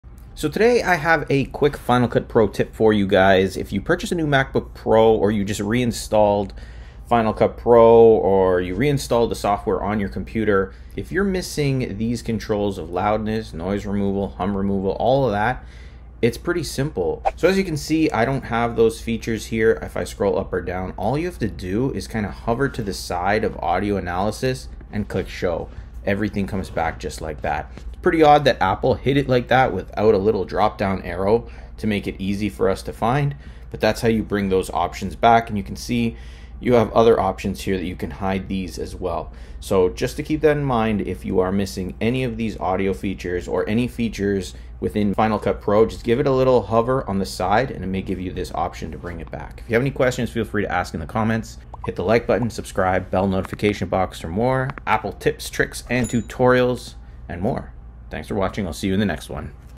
Final Cut Pro Noise Removal Feature Missing Fix - Loudness & Hum Removal
In Todays Final Cut Pro tip, I will show you how to get the final cut pro noise removal feature back. If the Final Cut Pro noise removal feature is missing, this is the fix. You will also learn how to get the Hum Removal and Loudness audio options back as well.

From the FCP audio editor, hover your mouse over the Audio Analysis option, and a "Show" option will appear on the far right. Click show, and that will bring back the noise removal feature in FCP. Hum Removal and Loudness will also be back.